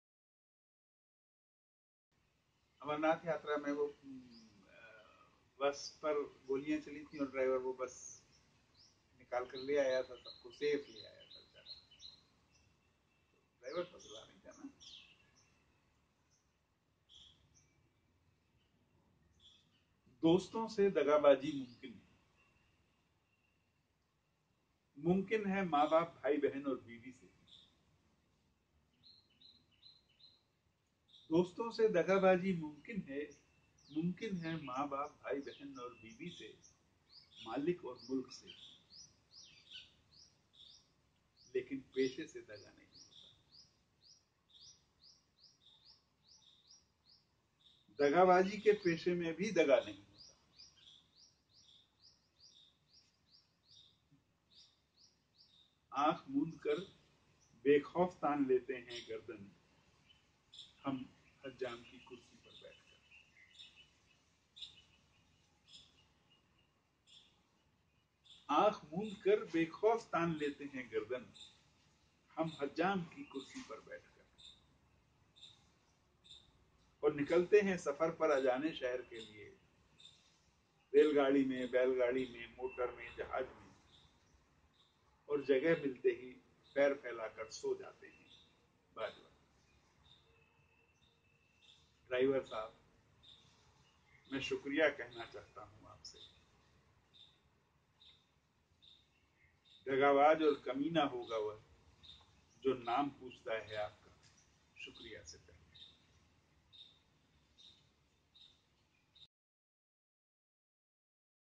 'Dagabaazi' Rajendra Sharma
Renowned Hindi poet Rajendra Sharma reads his poem 'Dagabaazi.'

To know more about Anjuman, follow us at -